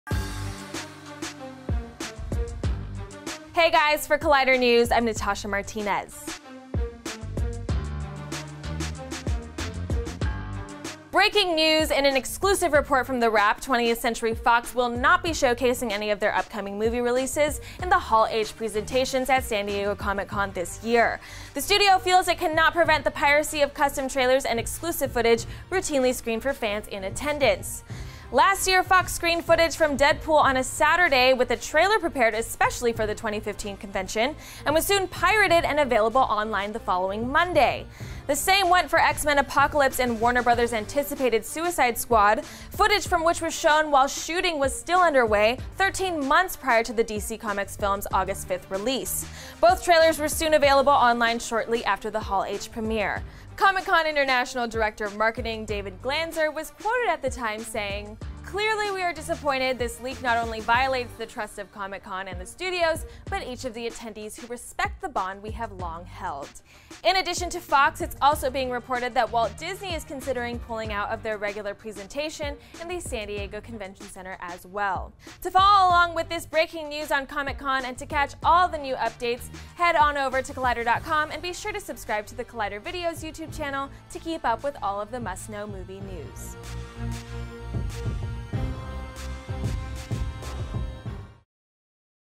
Collider News: 20th Century Fox Pulls out of Comic Con's Hall H Presentation
Natasha Matinez returns with Collider News! Because of all the pirated leaks from Comic Con's Hall H presentations, 20th Century Fox has pulled their movie presentation and will not appear at this years convention.